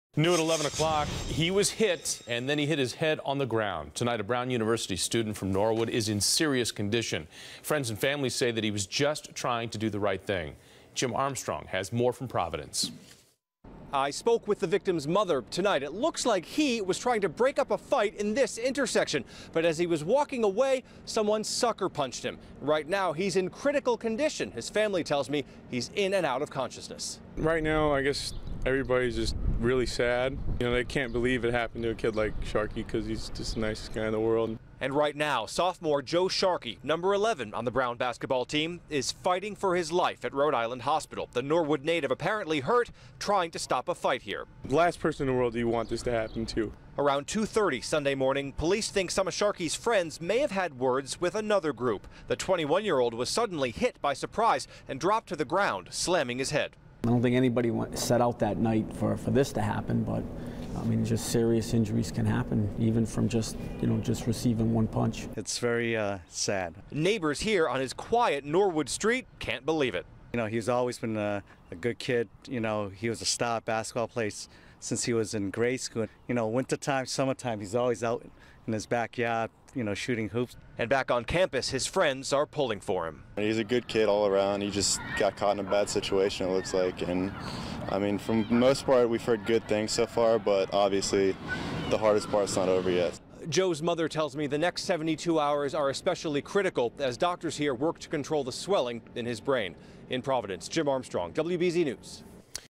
Norwood Man Critically Injured By Head Punch In Providence
Norwood Man Critically Injured By Head Punch In Providence watched by Mrtodayvideos2.